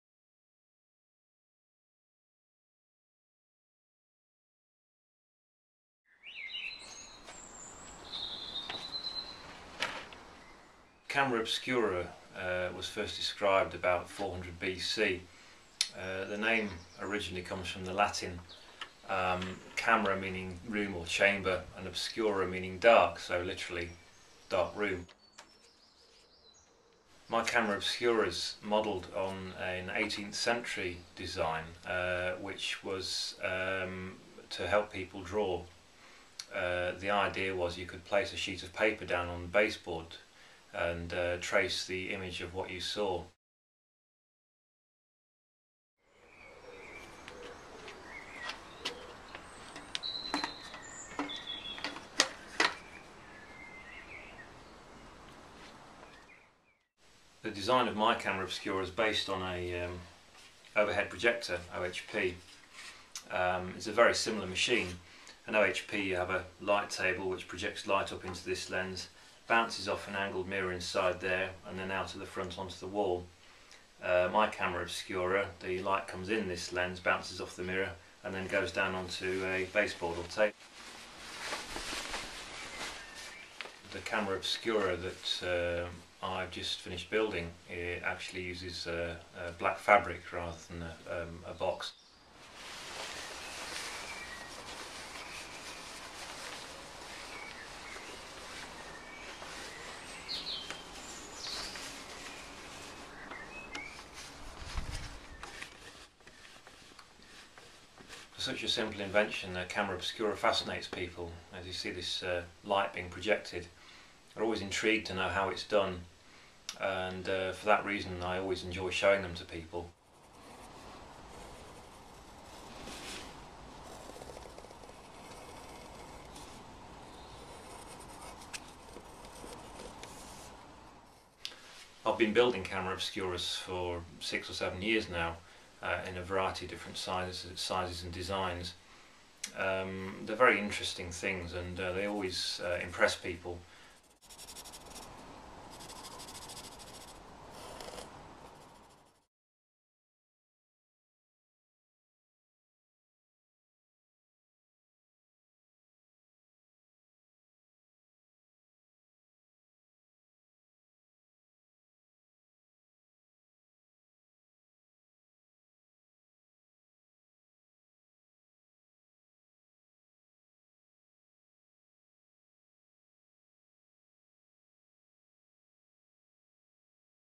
My new Camera Obscura
A brief film demonstrating a camera obscura I built recently